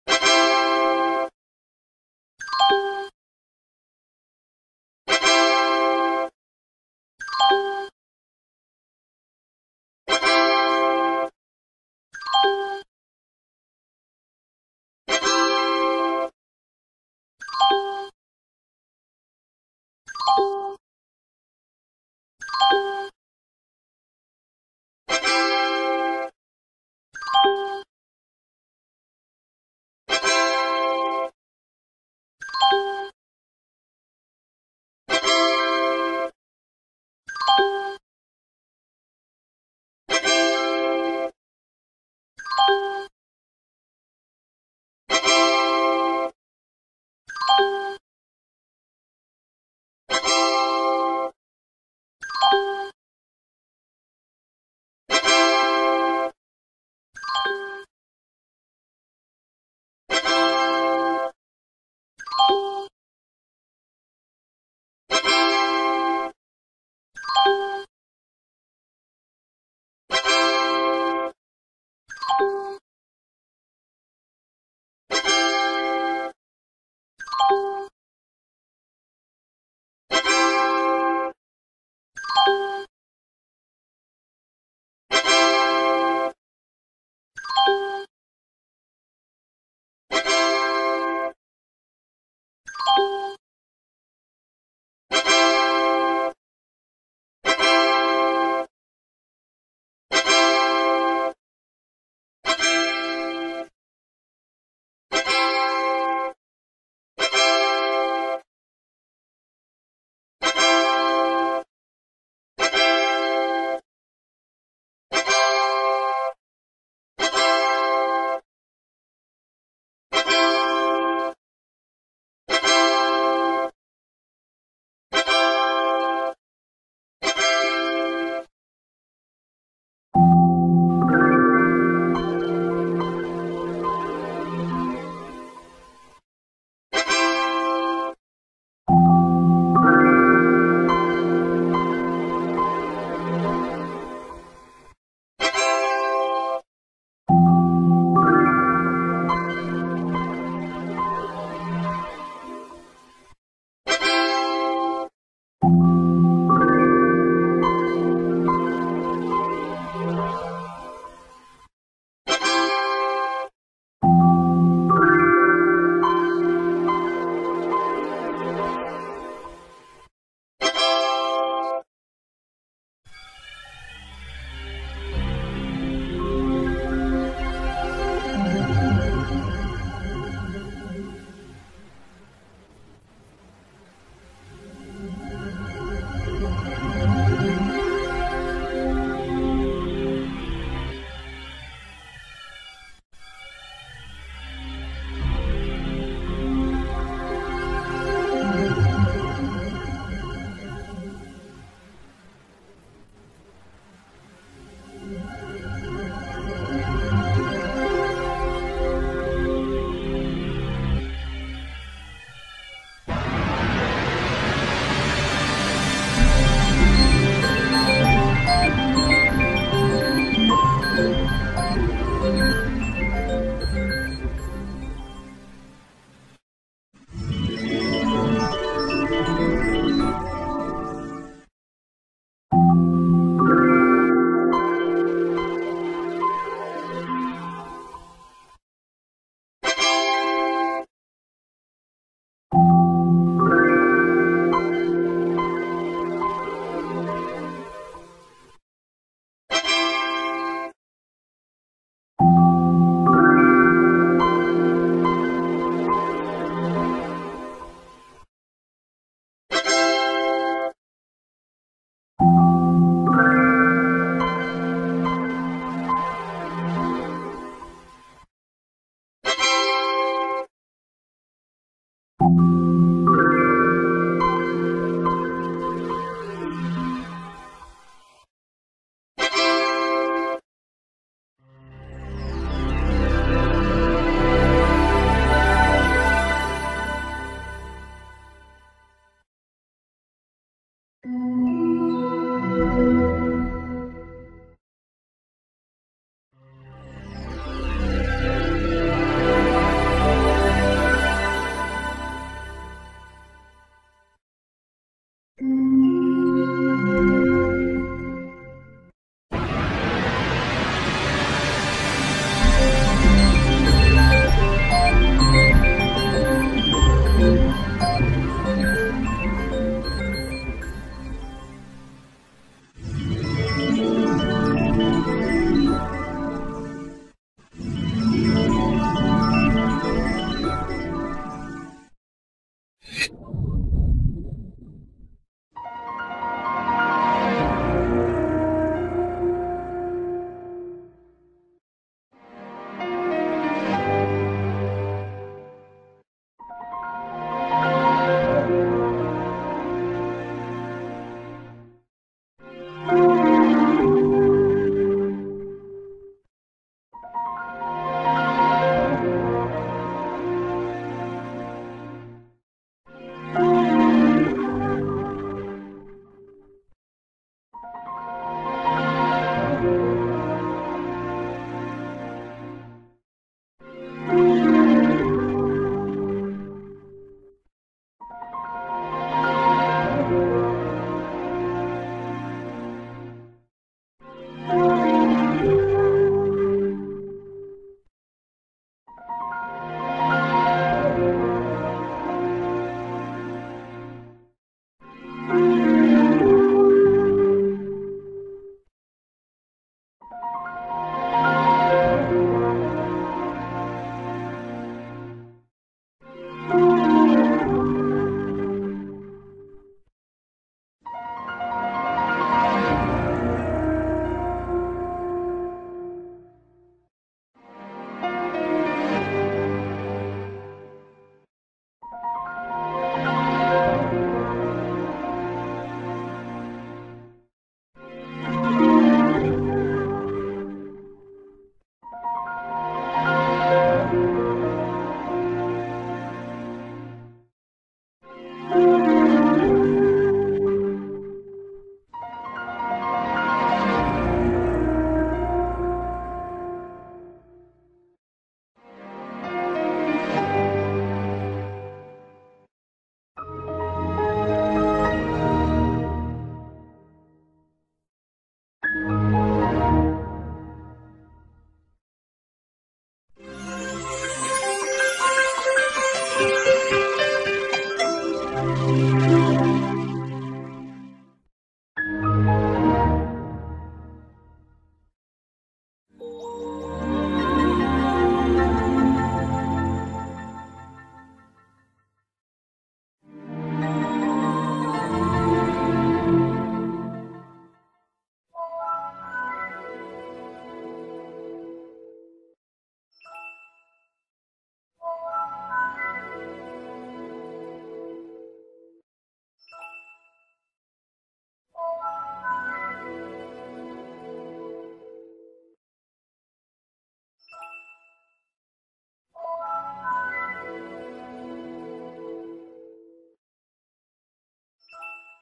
All Windows Startup And Shutdown Sounds
From Windows 1.0 To Windows 10.